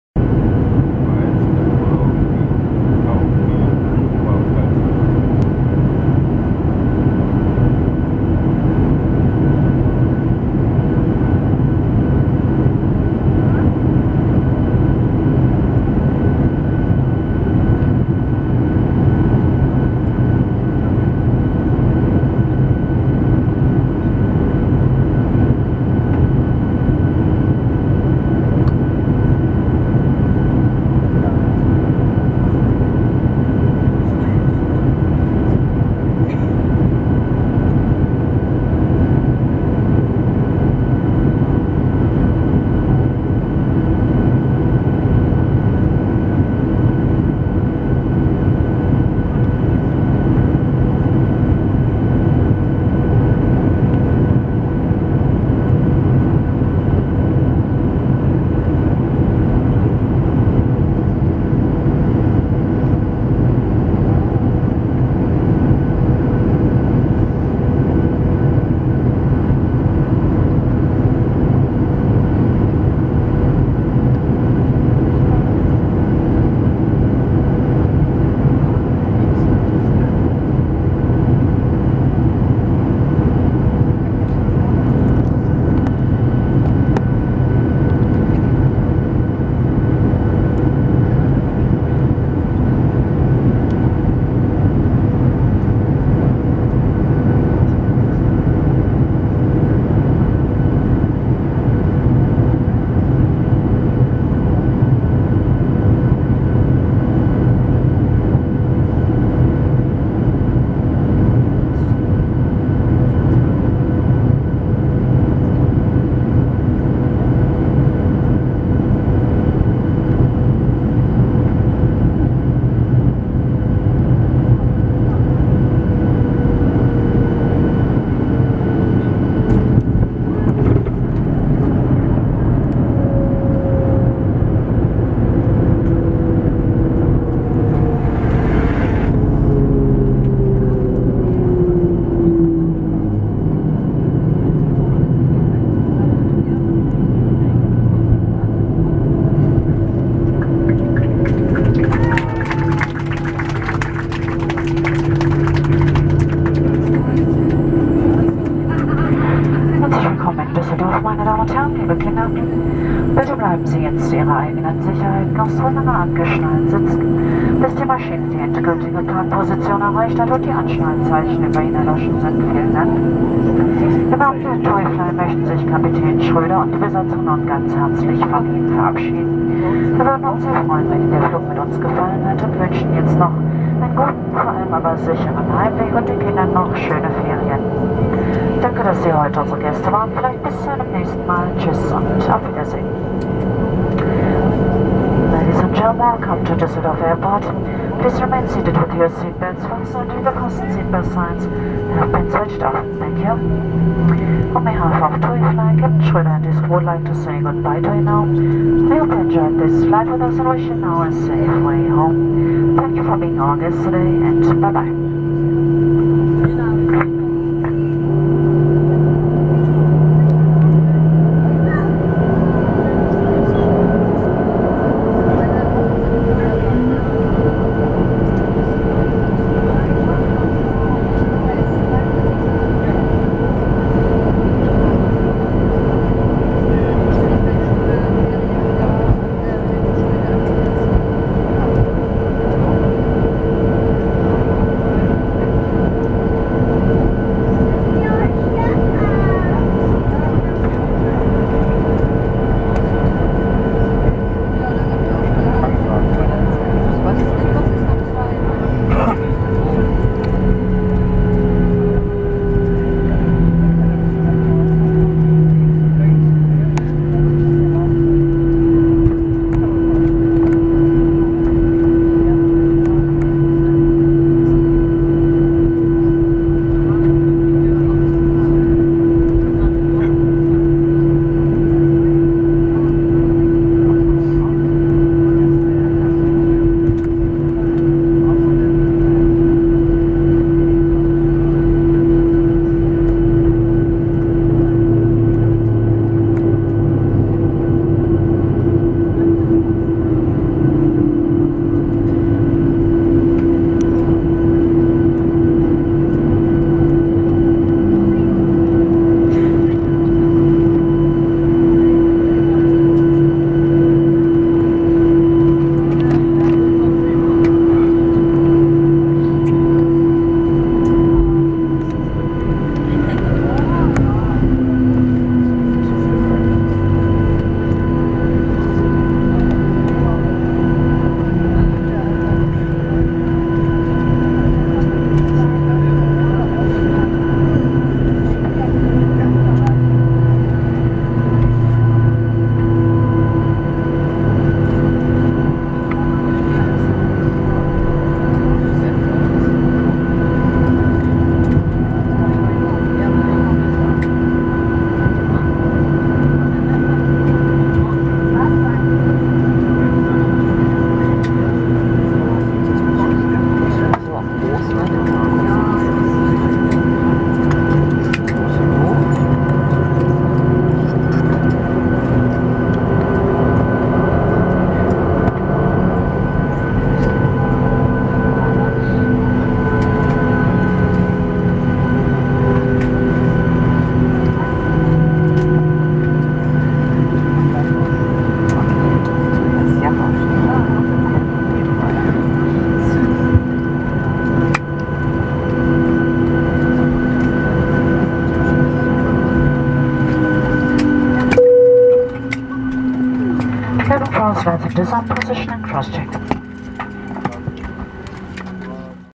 [FULL HD] TUIfly Boeing 737-800 Night Landing Düsseldorf Airport
TUIfly Boeing 737-800NG Night Landing at Düsseldorf International Airport. Enjoy!!! Please Comment and If you like this Video give me a Thumbs up ;)

Air Company: TUIfly
Aircraft: B737-800NG D-ATUD
DEP: HER, Heraklion, Greece
ARR: EDDL, Düsseldorf, Germany
Seat: 17 A
Flight number: X3 4105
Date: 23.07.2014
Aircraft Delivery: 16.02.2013
Weather: Clear skies, Dark , 21+C